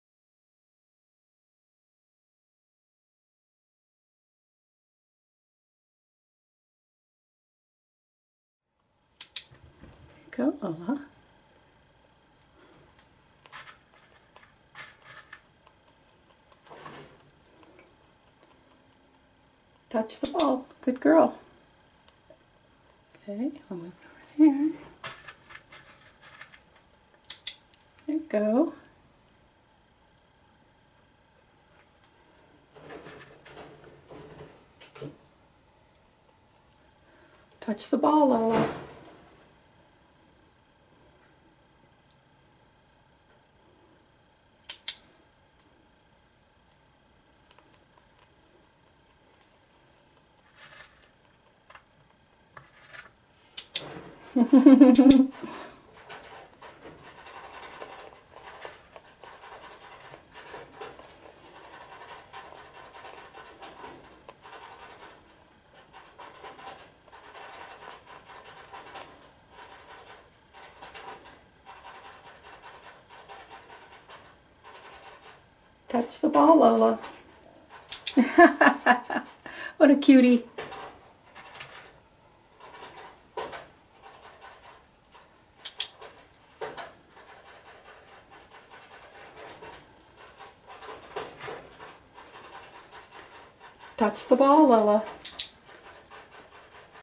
Clicker training Lola the Havana rabbit
I want to train Lola for Bunny Hopping and Rabbit Agility. I thought a good place to begin was using clicker training to teach her to follow a target. During this session, Lola began to touch the target (a practice golf ball).